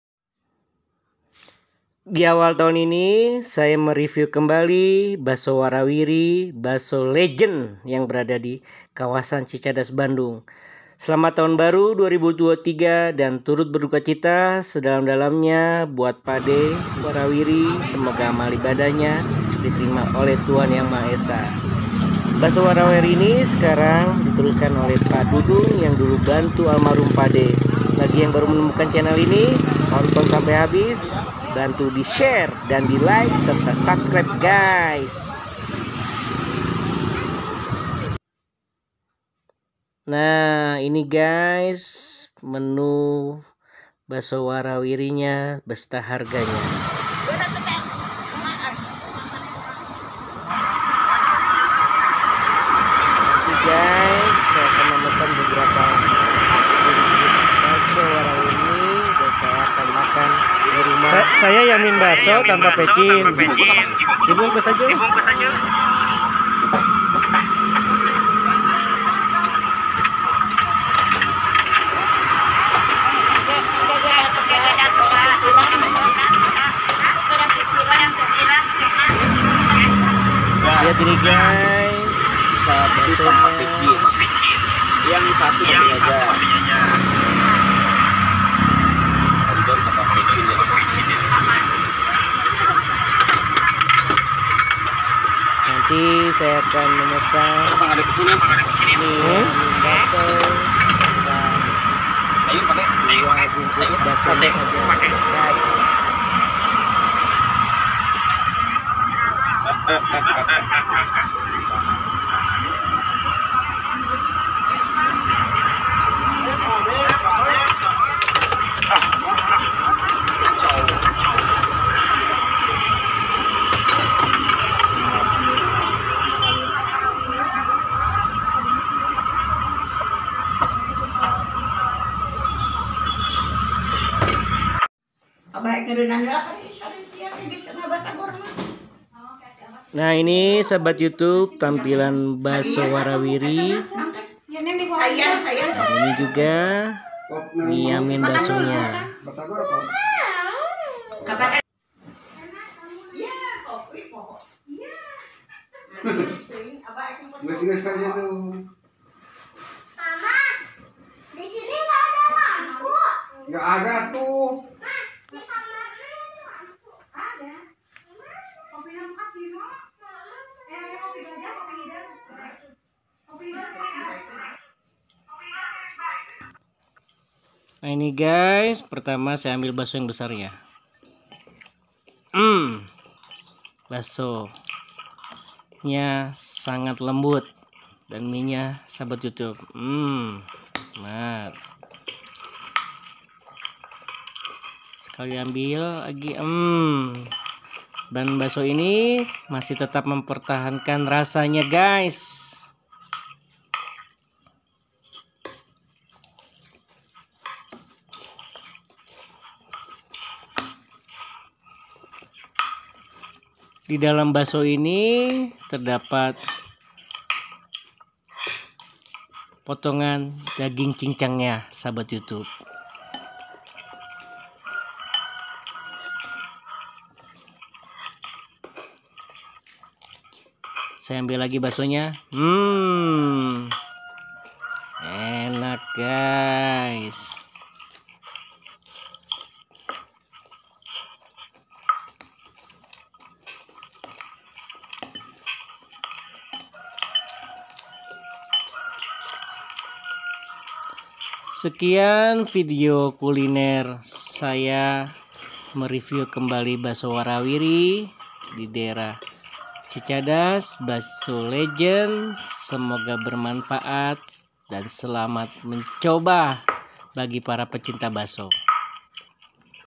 Mereview Terbaru Bakso Wara Wiri |Bakso Legend Cicadas Bandung
mereview kembali bakso wara wiri yang berada di Cicadas Bandung,saya mengucapkan turut berduka cita sedalam dalamnya almarhum pakde wara wiri semoga amal ibadahnya diterima Tuhan Yang Maha Esa